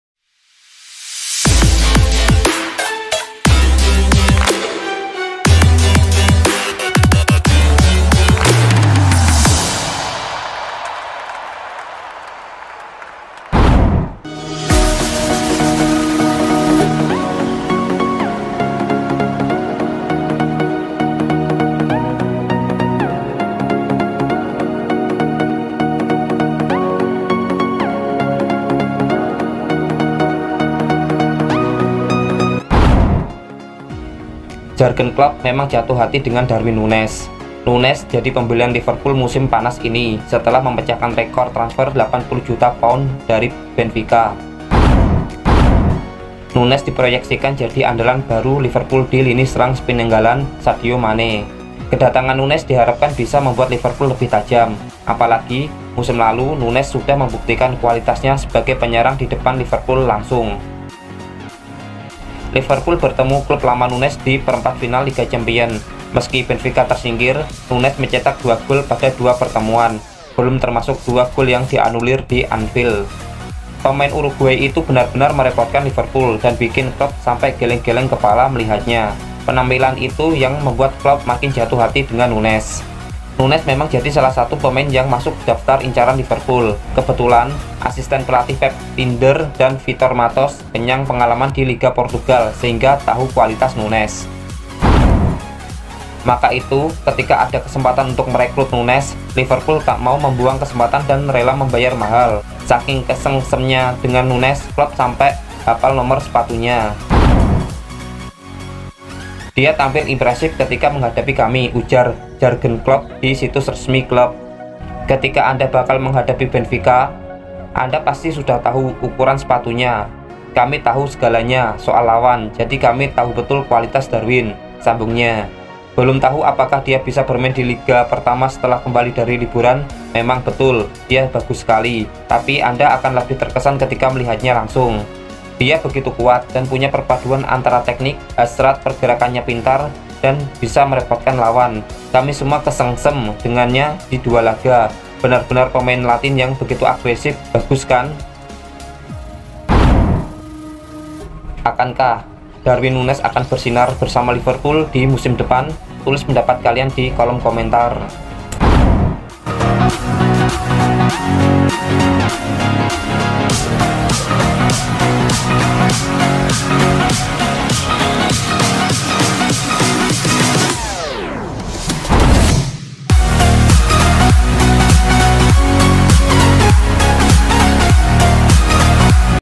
Juergen Klopp Jatuh Hati Kepada Darwin Nunez.
Juergen Klopp memang jatuh hati dengan Darwin Nunez.
Nunez jadi pembelian Liverpool musim panas ini setelah memecahkan rekor transfer 80 juta paun dari Benfica. Nunez diproyeksikan jadi andalan baru Liverpool di lini serang sepeninggal Sadio Mane.
Kedatangan Nunez diharapkan bisa membuat Liverpool lebih tajam. Apalagi musim lalu, Nunez sudah membuktikan kualitasnya sebagai penyerang di depan Liverpool langsung.
Bagaimana kah kelanjutannya???"

Ikuti terus channel ini, supaya tidak ketinggalan berita terbaru seputar dunia sepakbola.
thanks...